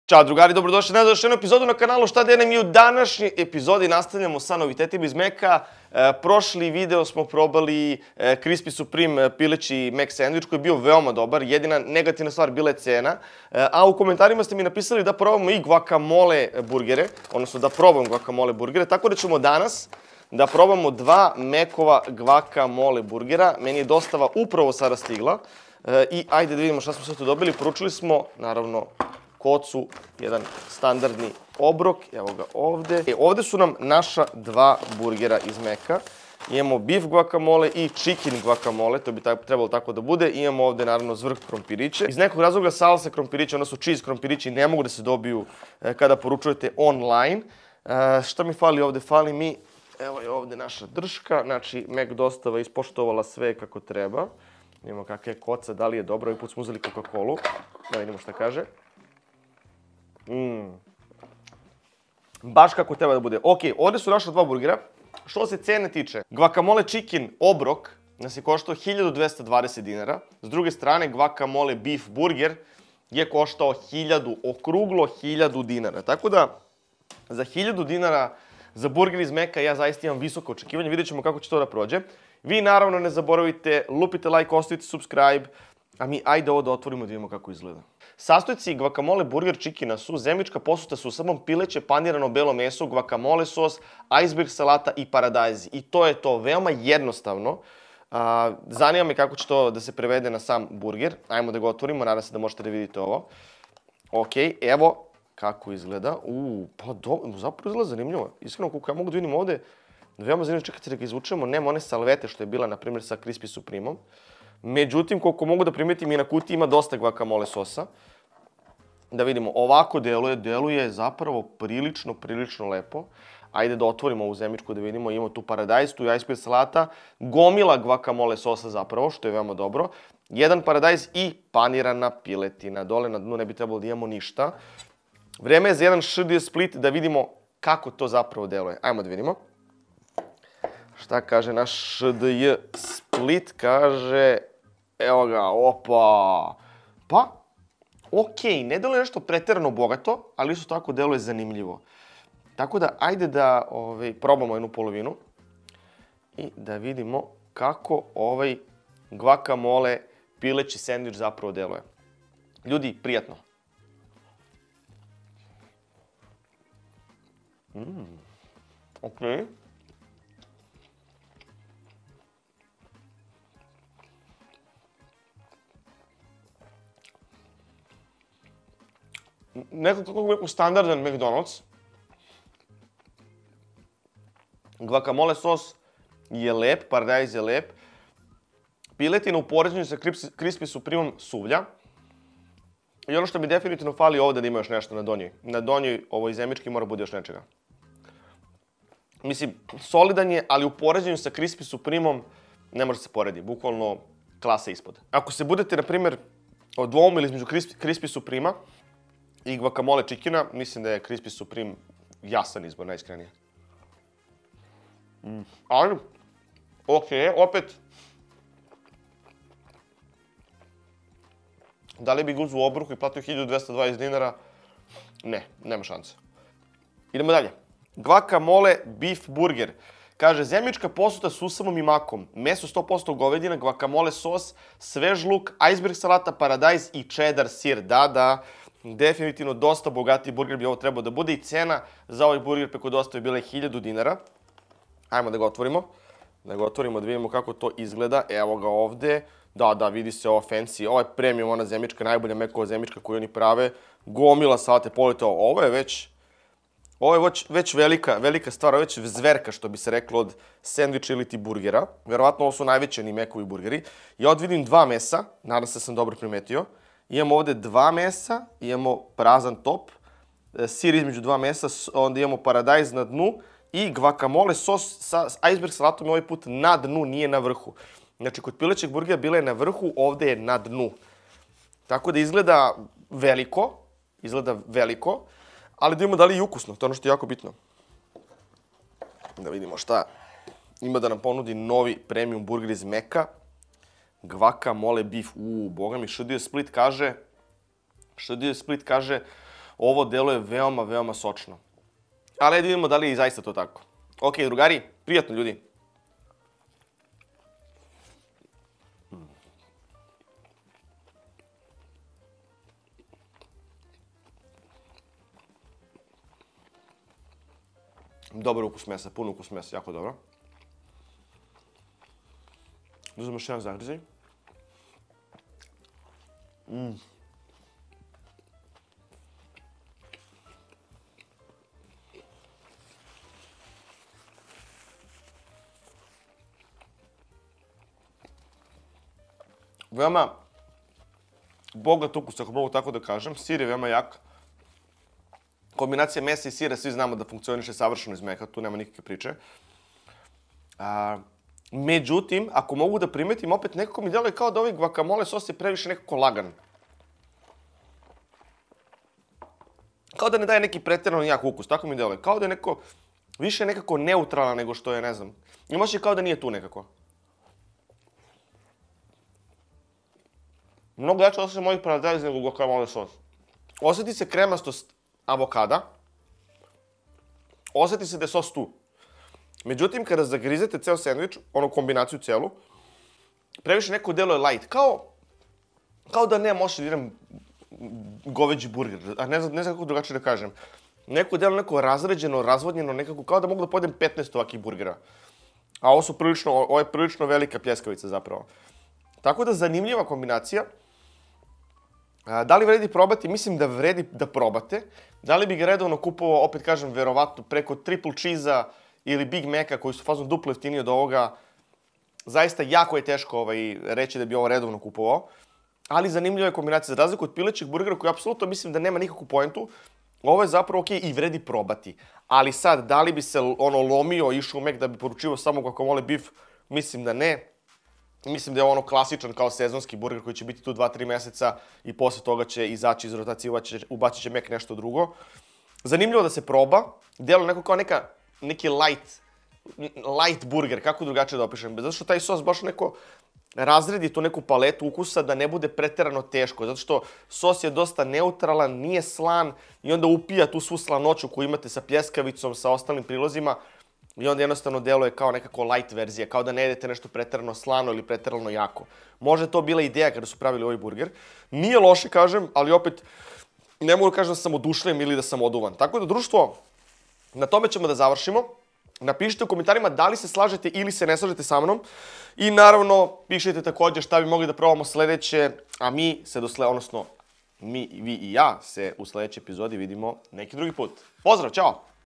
KAKVI SU GUACAMOLE CHICKEN I BEEF IZ MEKA?
U današnjoj epizodi nastavljam sa degustacijom novih burgera iz McDonalds restorana. Na red su došli Guacamole Chicken i Guacamole Beef. Cena ovih burgera nije najsjajnija ali sve će im biti oprošteno ukoliko ukus bude na visokom nivou. Obavezno napišite kako se vama dopadaju ovi novi burgeri ukoliko ste ih probali. :)

Ukoliko imate predloge šta bi mogli da probamo sledeće, ostavite komentar ispod :)